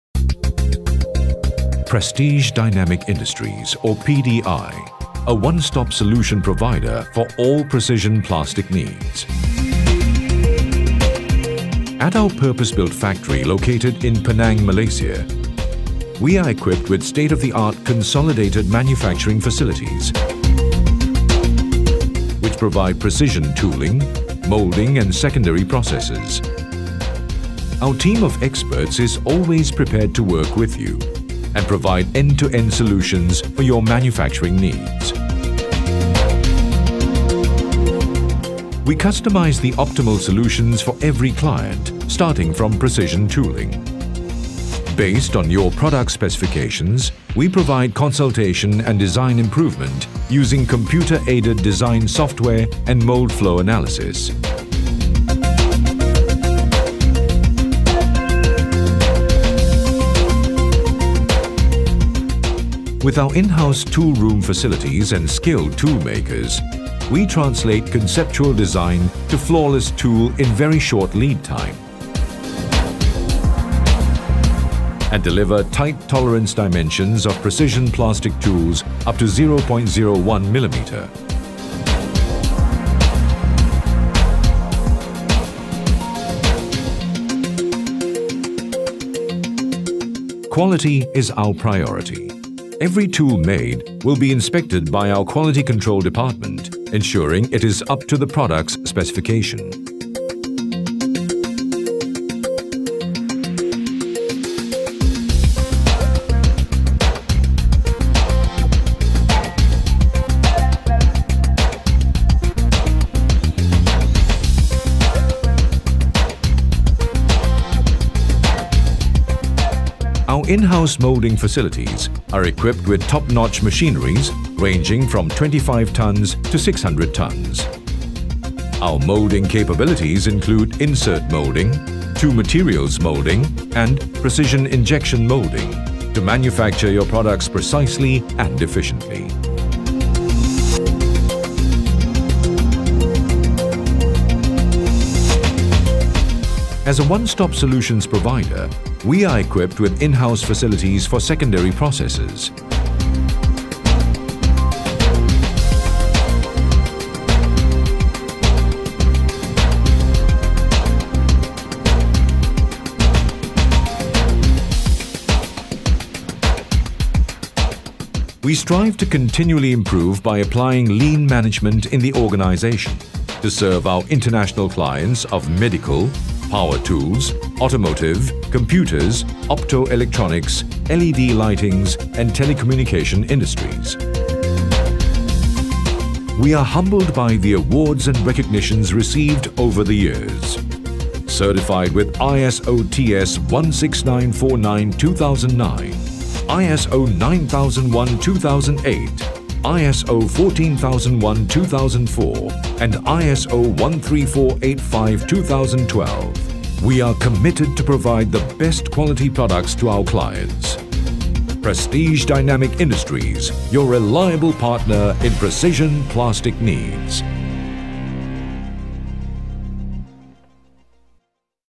Prestige Dynamics Industries Sdn Bhd
Welcome to Prestige Dynamics Industries

Prestige Dynamics Industries Sdn Bhd (PDI) has started its operations in May 1990 and aspires to be one of the leading one stop centres for plastic injection moulding industry. We specialize in precision mould making, custom injection moulding and secondary processes. All manufacturing takes place in our fully integrated 10,000 square meters manufacturing plant, located in Penang, Malaysia. The company consists of approximately 350 employees.

Being an ISO/TS 16949 certified company; PDI has gained global recognition as an established and reputable supplier which supplies quality products with the most stringent requirement of the automotive industries. For over 23 years, our existing customers have strongly recommended PDI as the preferred supplier with excellent performance in quality, delivery and services; allowing PDI to enjoy rapid growth in its new customer base. Influx of sales from the new customers had required us to continuously expand our capacity to cater to new demand.

Our Vision
To be an internationally recognized one stop service centre for all your precision plastics needs.

Our Mission
We are committed to providing the optimal plastic solutions services through our dedicated professional workforce and continual improvement in quality.